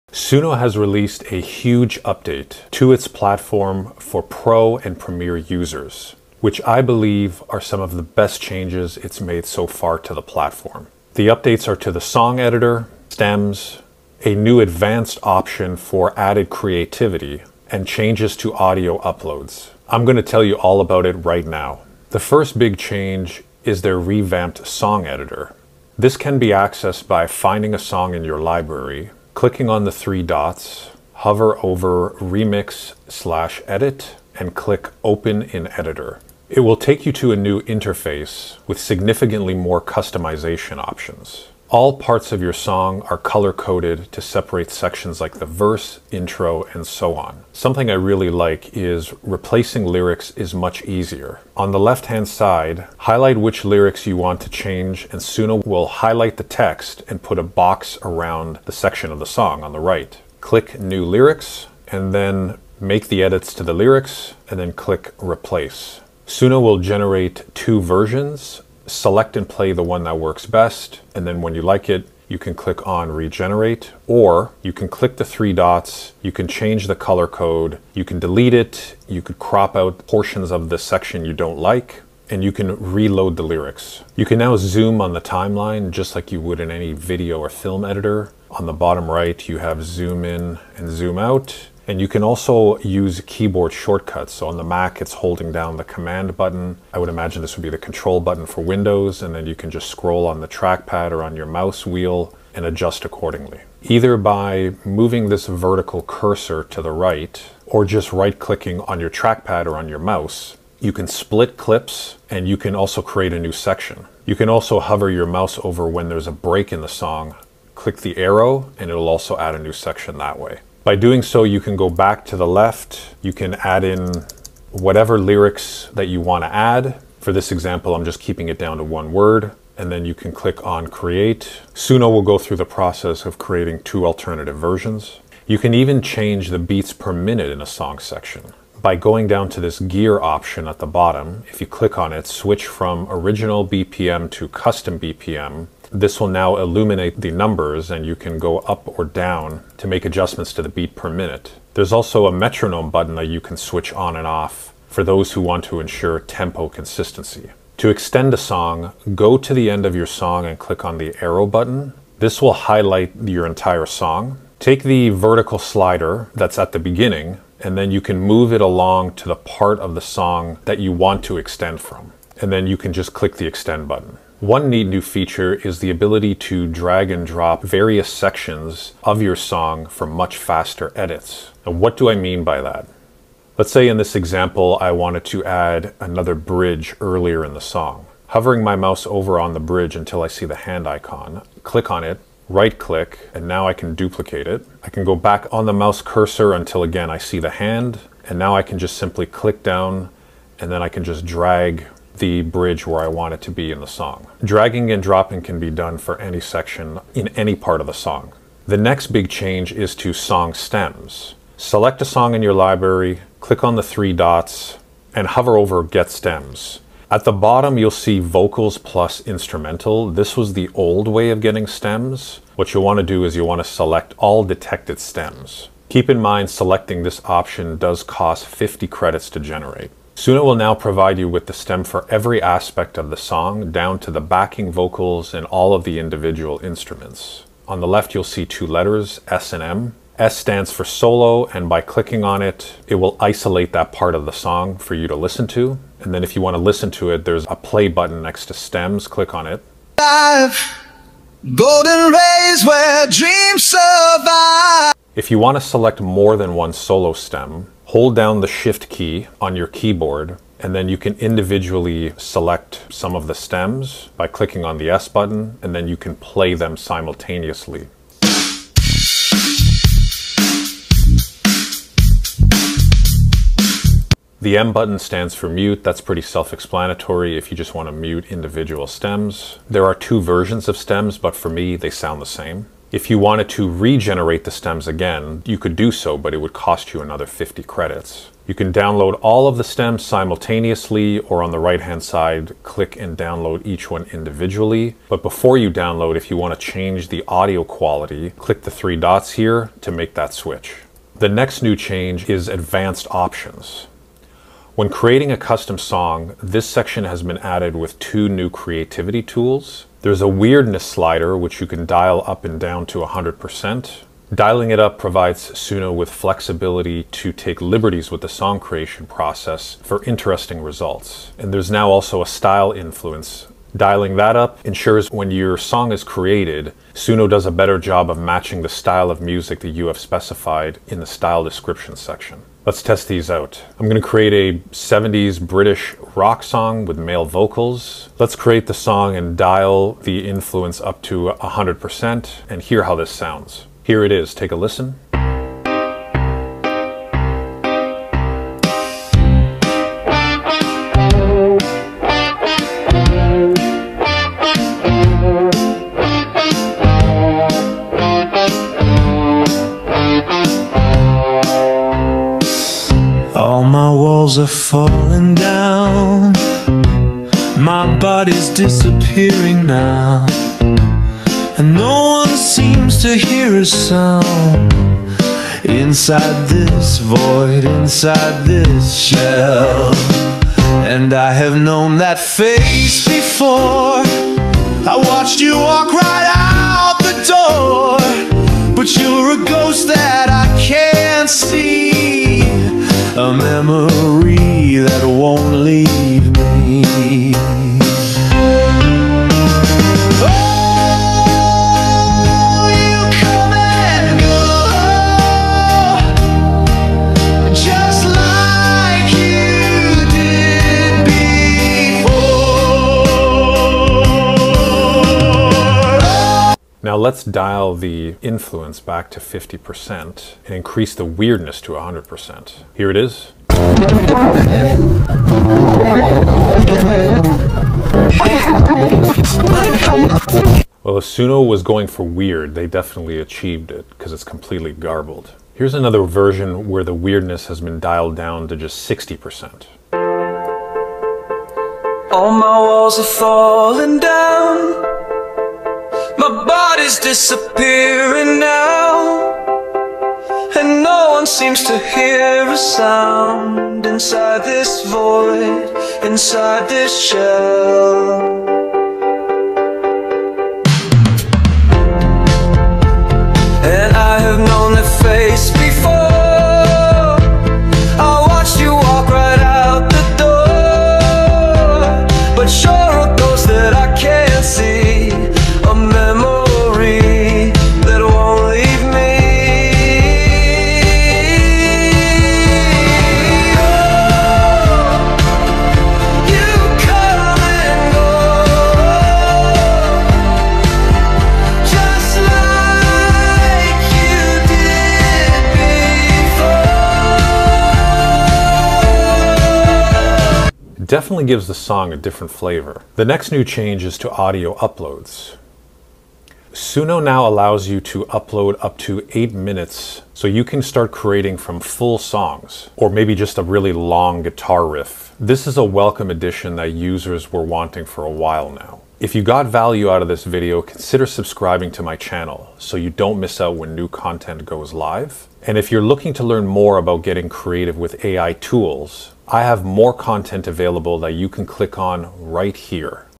Suno AI's Brand NEW Update: Everything You Need To Know!
Discover the groundbreaking capabilities of Suno AI's new song editor and advanced options features!

In "Suno AI's Brand NEW Update: Everything You Need To Know!", this video dives into the innovative features that set this AI music generator apart from the rest.

Learn how to use Suno AI new song editor, advanced options, revamped stem downloads and song upload process to add seamless song endings and streamline your editing process.

00:00 What’s New?
00:25 How to Use Suno 4.5 Song Editor
00:51 How to Replace Song Lyrics in Suno AI
01:24 How to Zoom In and Out using Suno Song Editor
01:43 How to Split Suno Song Clips
01:54 How to Add New Song Sections in Suno 4.5 AI
02:15 How to Change Suno Song Beats Per Minute
02:37 How to Extend Songs Suno AI 4.5
02:54 How to Drag and Drop Parts of a Song Suno
03:31 How to Get Suno AI Song Stems
05:03 How to Use Suno Advanced Options
05:33 How to Use Suno Style Influence
06:56 How to Use Suno Weirdness Slider
08:26 How to Upload Audio to Suno